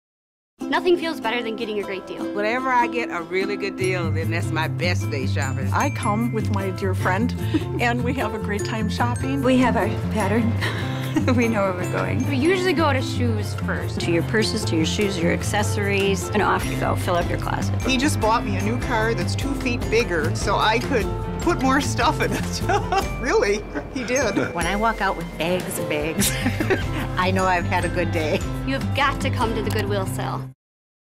Carson's Goodwill Sale: You Gotta Come
The Goodwill® Sale  March 10 thru 24, 2010
The Goodwill coupon is good towards merchandise that rarely, if ever, go on saleincluding cosmetics.  Your donation is passed along to Goodwill Industries to provide training, employment and supportive services for people with disabilities or disadvantages who seek greater independence